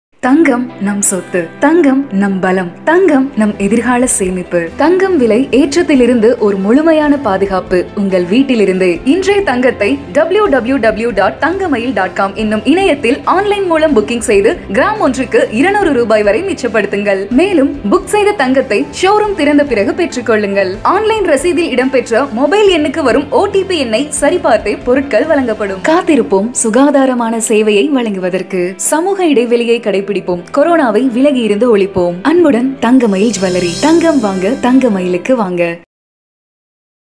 Thangamayil Jewellery - Freeze Your Gold Rate | Pre-Book Gold Jewellery | Akshaya Tritiya | 2020
This Akshaya Tritiya book your Gold Online & get upto Rs.200 OFF per gram*T&C apply. Stay Home & Stay Safe.